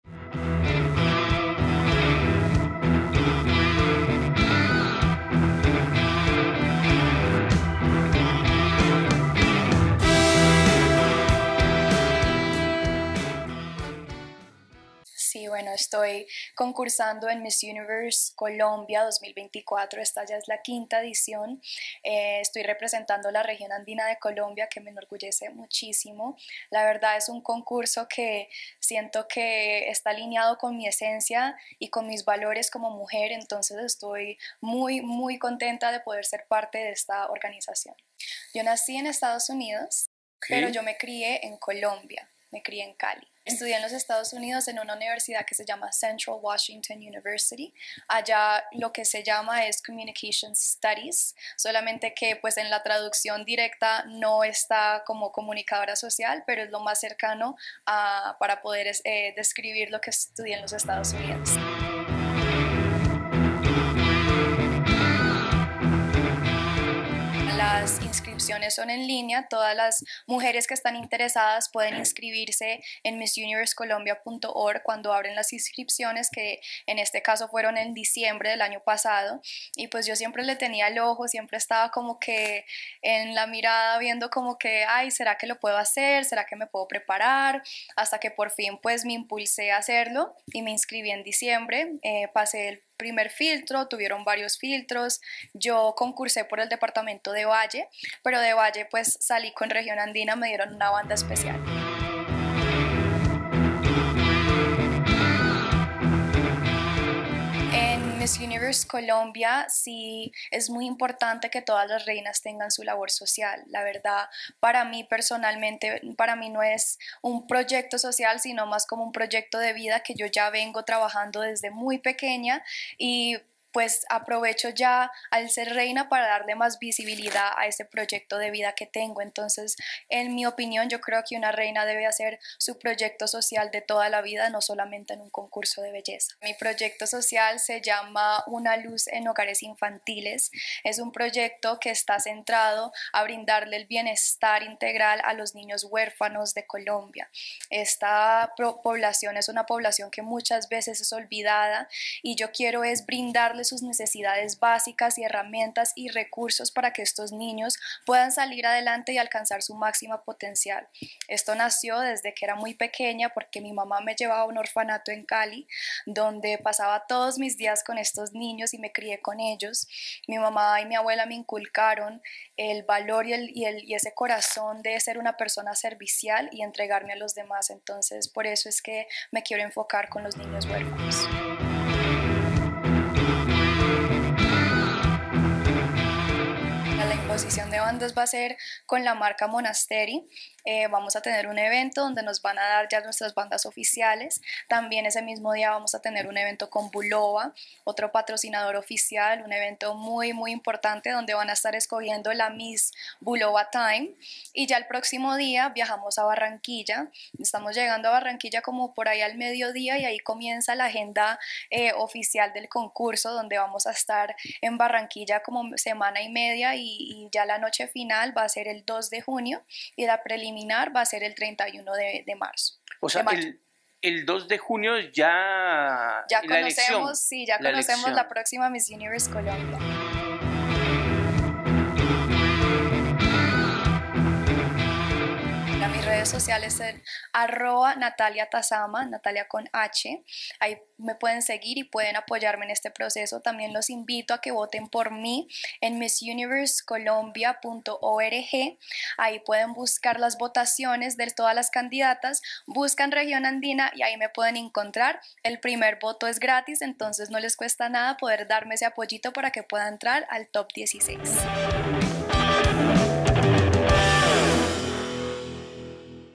Nathalia Tasama, Aspirante a Miss Universe 2024
Nathalia Tasama, una joven de 24 años, emerge como una figura destacada en el ámbito del compromiso social y la comunicación. Con una sólida formación académica y una dedicación inquebrantable a causas humanitarias, su impacto se siente tanto a nivel local como internacional. Actualmente, Nathalia está participando en Miss Universe Colombia 2024, representando a la región andina, compuesta por 13 departamentos.

Acompáñanos a conocer su inspiradora historia y su compromiso con la comunidad. ¡No te pierdas su trayectoria hacia la corona de Miss Universe 2024!
❤️👇SÓLO PARA AMANTES DE LA MÚSICA, MODA , ENTRETENIMIENTO Y CULTURA:
Si quieres generar una entrevista, te recomiendo que hagas click en este enlace 👉👉

Bogotá - Colombia